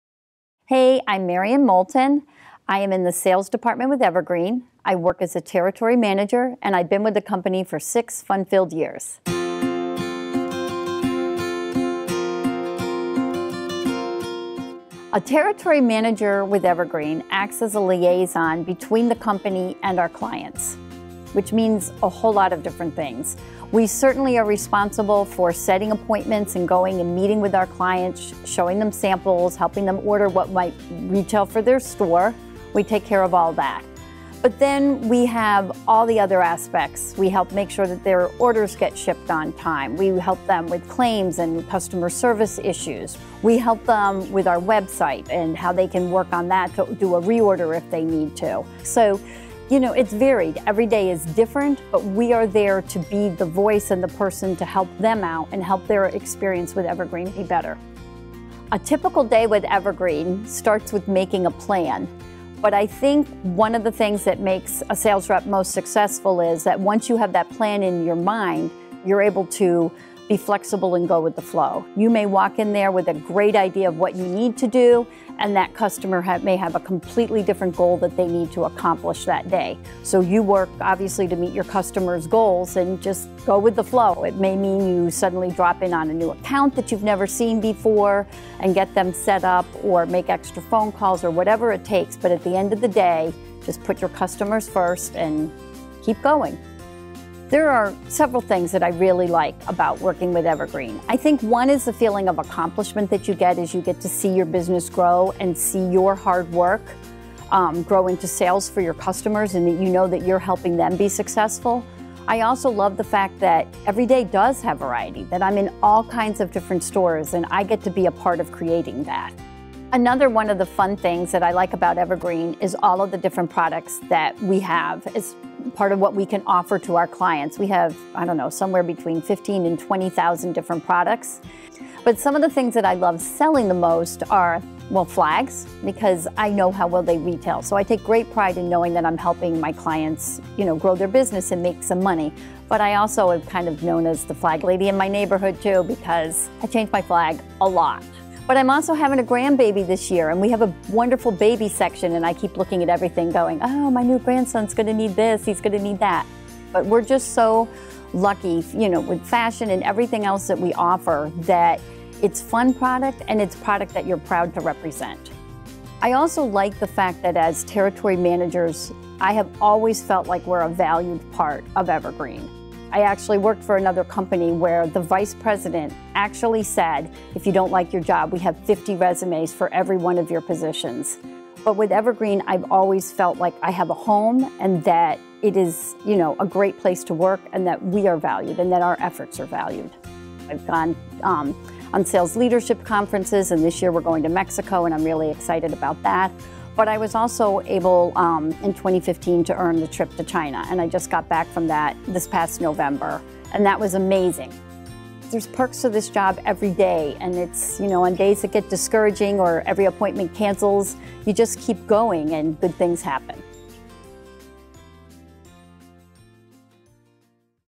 Employee Spotlight - Marion Moulton (Territory Sales)
To help support our thousands of retail customers, we have team of territory sales managers, located all over North America. Get an "inside look" into what it's like to be part of the Evergreen sales force in our latest Employee Spotlight.